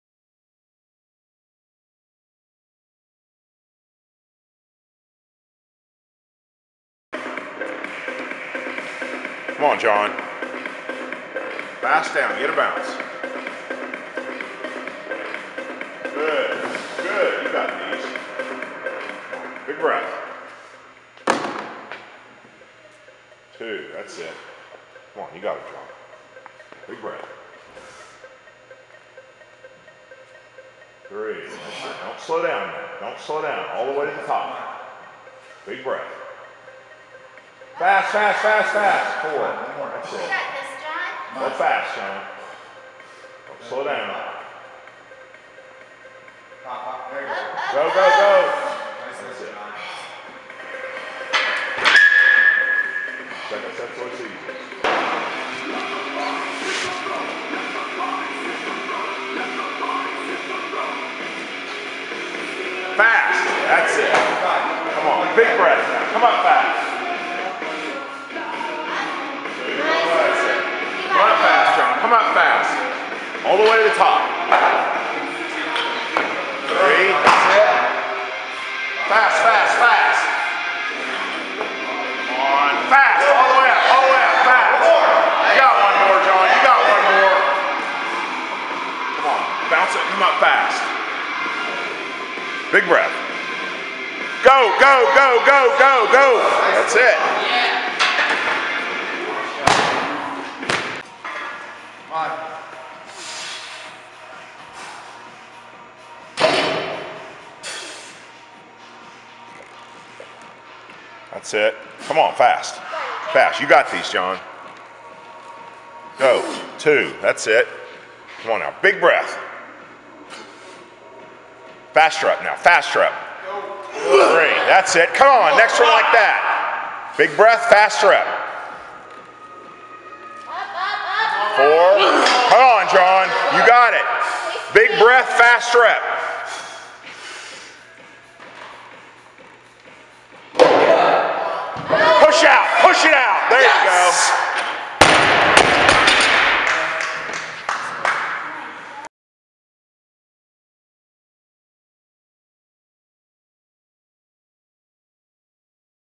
Jon does some more squats.
Jon squatted with maximal effort and minimal whining.  That alone was worth a video, the PR was just a bonus.